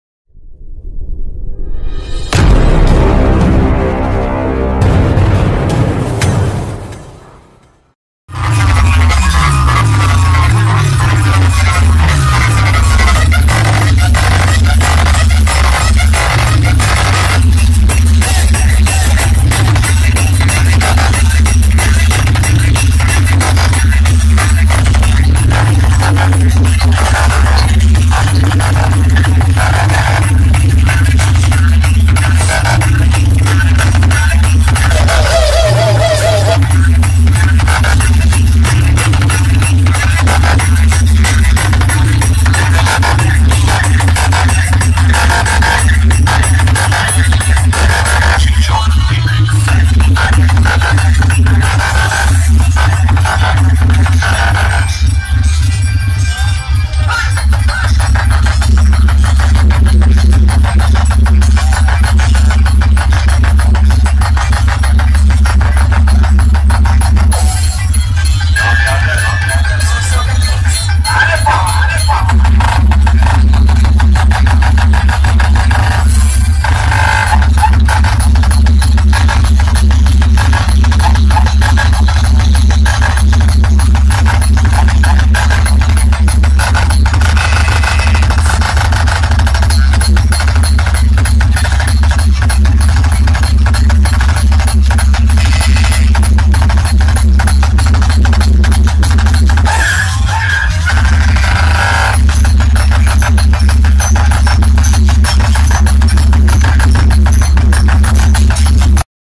BALASORE BOX COMPETITION 2023 DJ KALPANA VS DASABHUJA DJ AT BALASORE GANESH PUJA BHASANI 2023
ଜୟ ଜଗନ୍ନାଥ 🙏.

ଦୟା କରି ଭିଡିଓ କୁ ଶେଷ ପର୍ଯ୍ୟନ୍ତ ଦେଖନ୍ତୁ !🙏🙏🙏